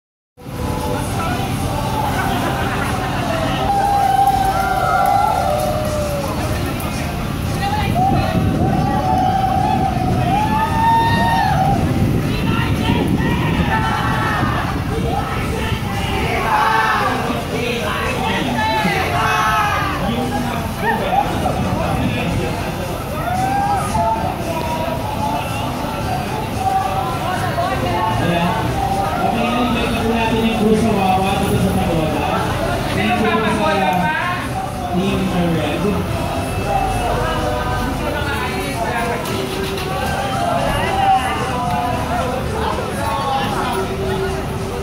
Ang Pagsayaw sa Mahal na Poong Krus sa Wawa (Grand Visitacion I )
Ang pagsayaw sa Mahal na Poong Krus sa Wawa ng Red Team.
10th Year Anniversary of Esculturas Religiosas En Las Filipinas
El Dia Grande En Bocaue